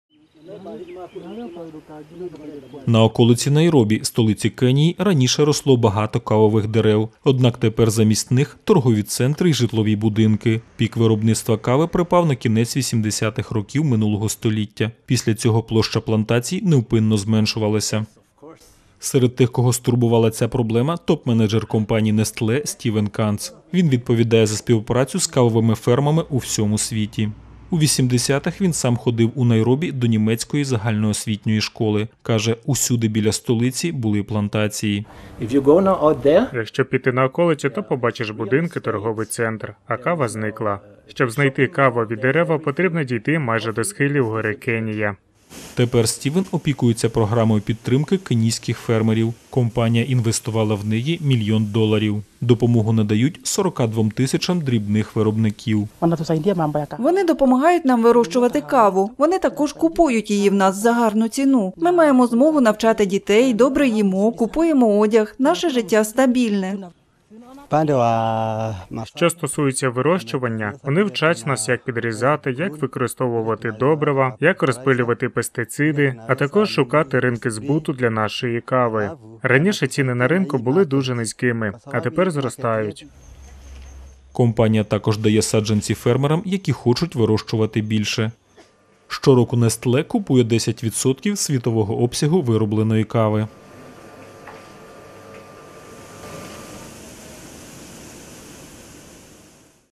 В Кенії замість кавових дерев будують житлові будинки
На околиці Найробі, столиці Кенії, раніше росло багато кавових дерев. Однак тепер замість них — торгові центри й житлові будинки. Пік виробництва кави припав на кінець 80-х років минулого століття. Після цього площа плантацій невпинно зменшувалася.

Серед тих, кого стурбувала ця проблема, — топ-менеджер компанії Nestle Стівен Канц. Він відповідає за співпрацю з кавовими фермами у всьому світі.

У 80-х він сам ходив у Найробі до німецької загальноосвітньої школи. Каже: усюди біля столиці були плантації.

Тепер Стівен опікується програмою підтримки кенійських фермерів. Компанія інвестувала в неї мільйон доларів. Допомогу надають 42 тисячам дрібних виробників.

Компанія також дає саджанці фермерам, які хочуть вирощувати більше.

Щороку Nestle купує 10 % світового обсягу виробленої кави.